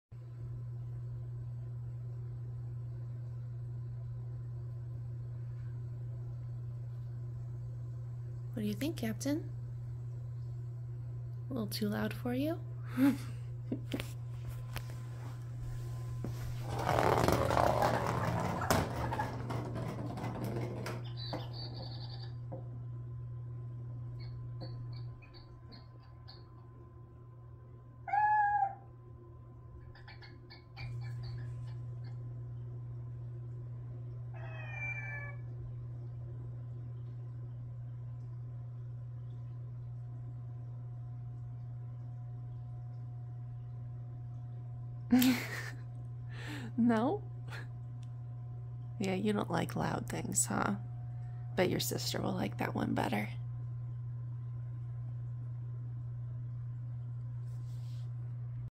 The Noisiest Cat Toy, Part 1: Captain in shock!
This one was a gift from a work friend.  Captain responds with a certain amount of disdain